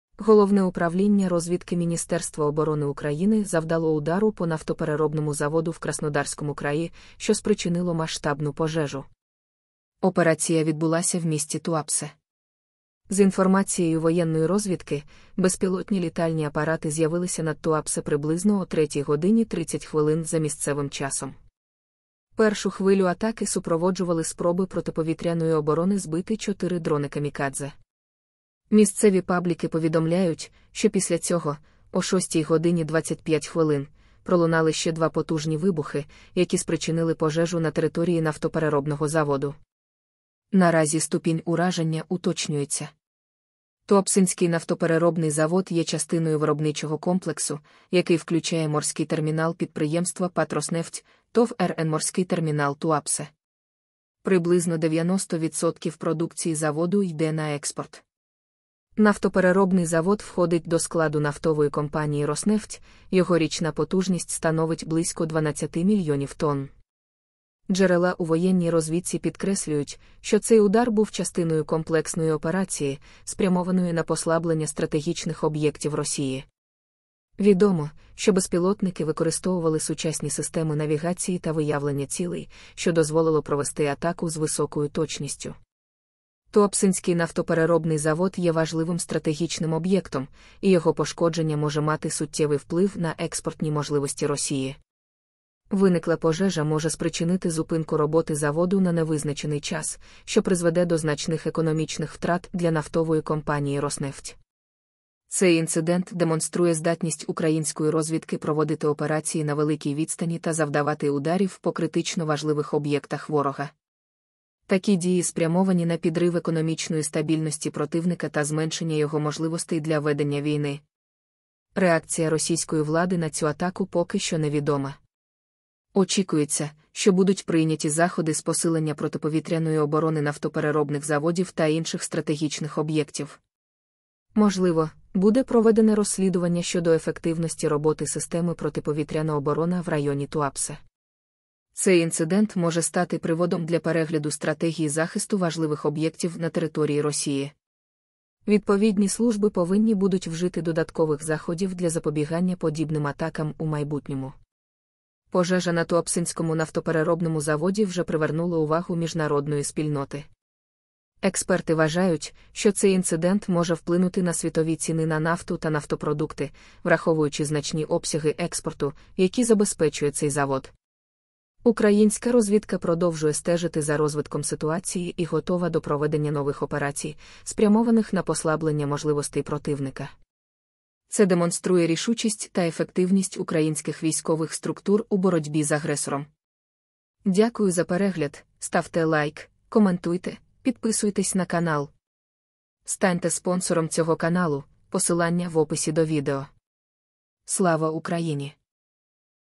Безпілотники завдали удару по НПЗ в Краснодарському краї РФ
• Фото матеріали використано з Вікіпедія або ж Фотобанків та Відкритих Джерел.